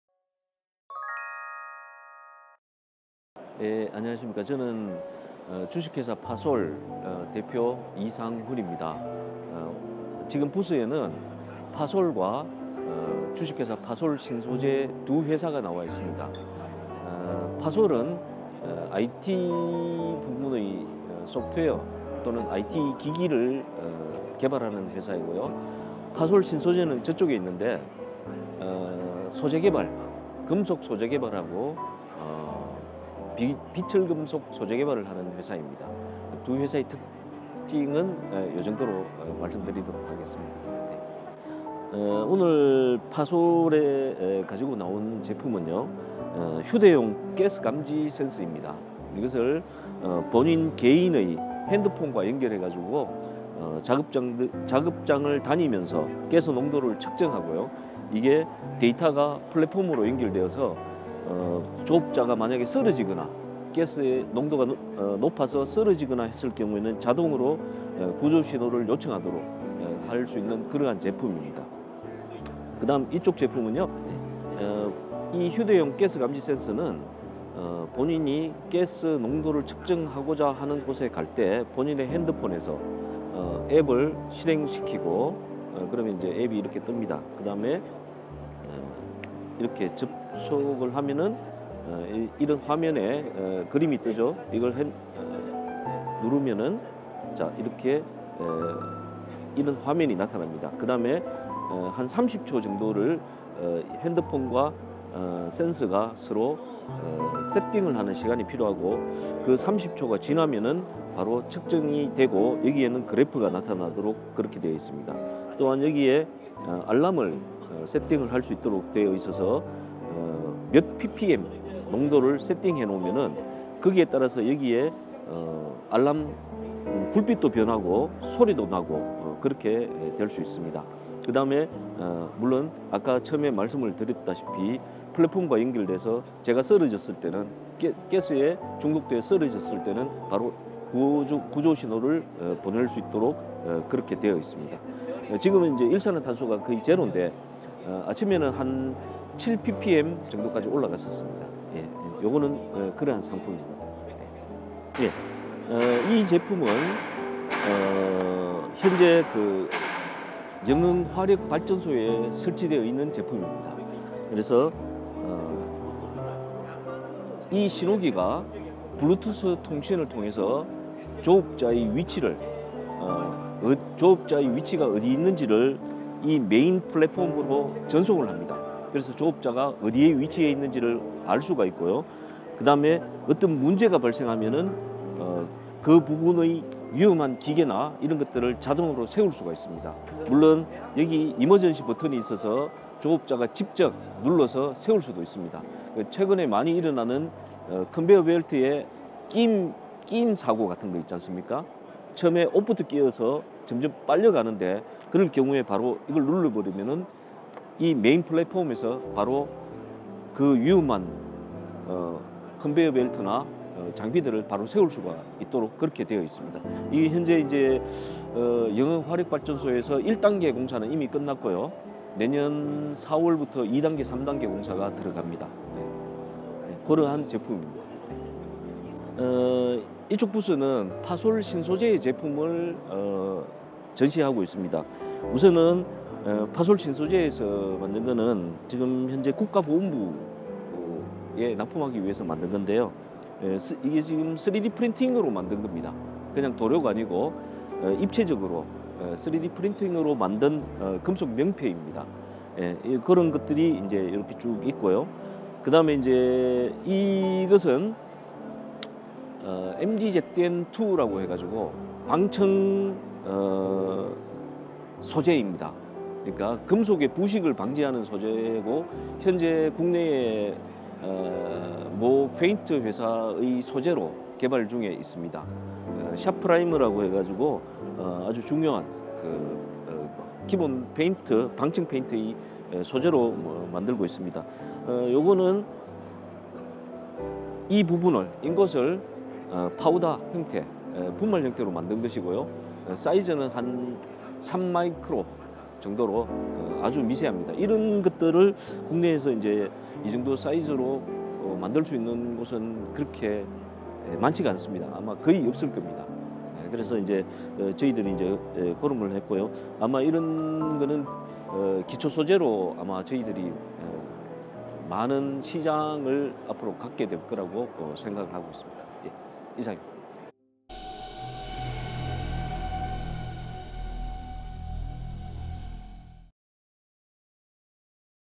[PARTS SHOW 2023] 언제 어디서나 갖고 다닐 수 있는 파솔의 ‘휴대용 일산화탄소 경보기’ 눈길
스마트팩토리 분야의 핵심 기술을 개발하는 파솔과, 금속 및 비금속의 신소재를 개발하는 파솔신소재를 소개합니다.